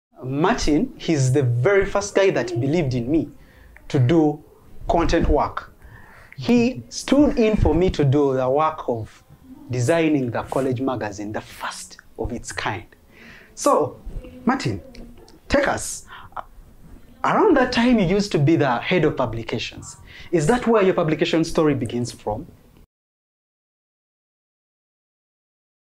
PEOPLE & IDEAS
our guest is Lubega Martin, the CEO ofy Health Publications...
Full clip coming soon